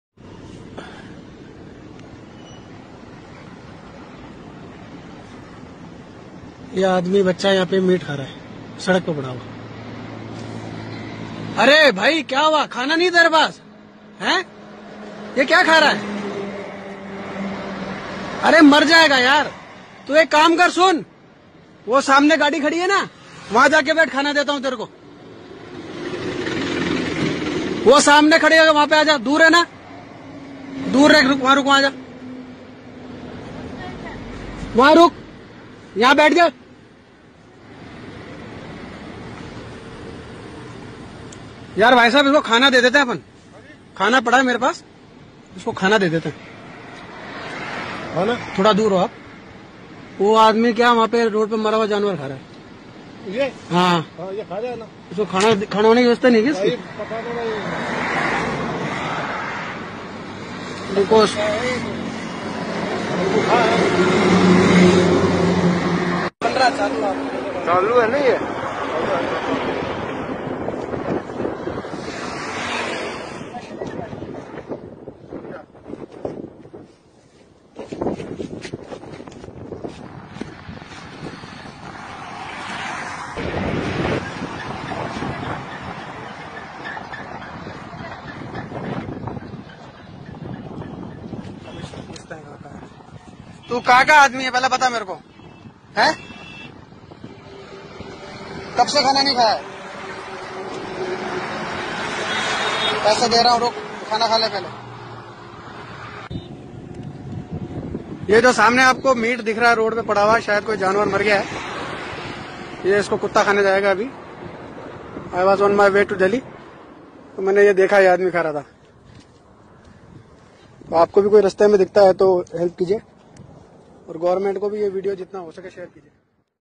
दिल्ली जयपुर नेशनल हाईवे पर मरा जानवर क्यों खाया इस शक्स ने..आगे जो हुआ आप सोच भी नहीं सकतेRajsthan
राजस्थान के शाहपुरा में दिल्ली जयपुर नेशनल हाईवे पर इंसानियत तब शर्मशार हों गई जब भूख के मारे एक व्यक्ति ने सड़क पर पड़े हुए मेरे जानवर कों खाना शुरू कर दिया..उसके बाद गाड़ी में वहां से गुजर रहे एक शकस ने उस आदमी को खाना खिलाया,पानी पिलाया और कुछ पैसे देकर उसकी मदद की